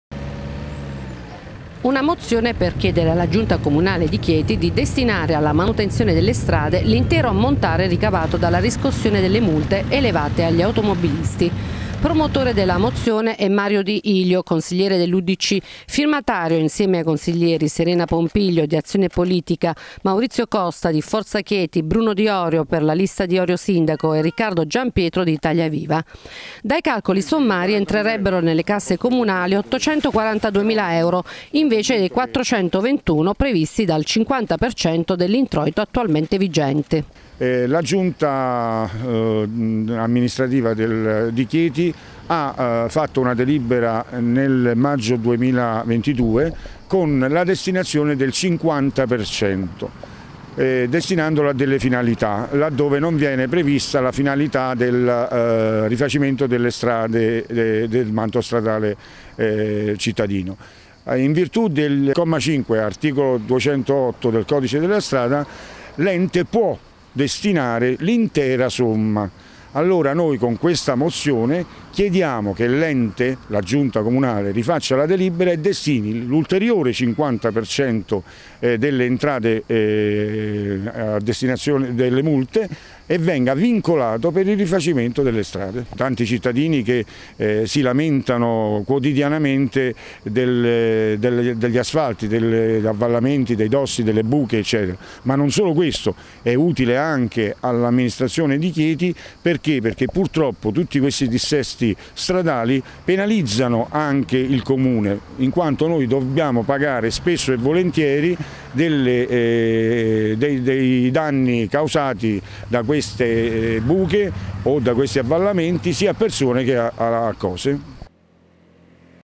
Chieti. Mozione per sistemare le strade
Destinare il pagamento delle multe elevate agli automobilisti alla sistemazione le strade cittadine e periferiche. Lo chiedono cinque consiglieri al comune di Chieti